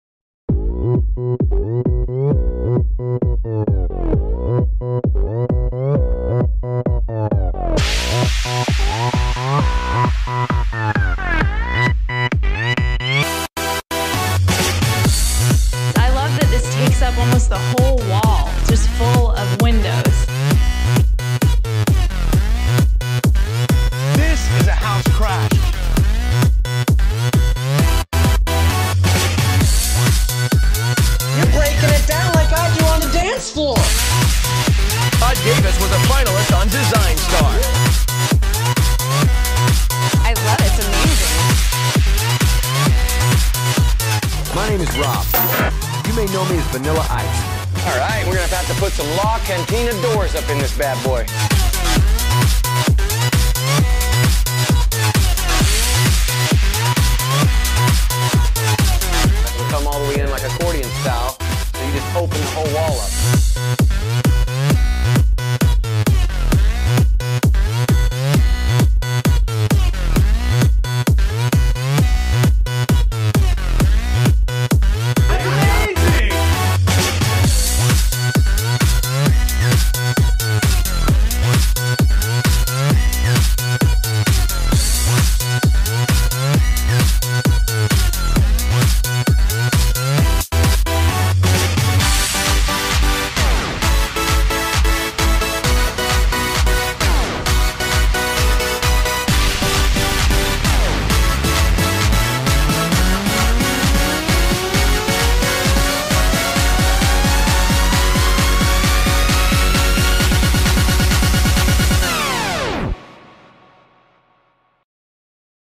LaCantina Doors on TV
Highlights from home improvement shows where the manufacturer's folding door systems have been featured.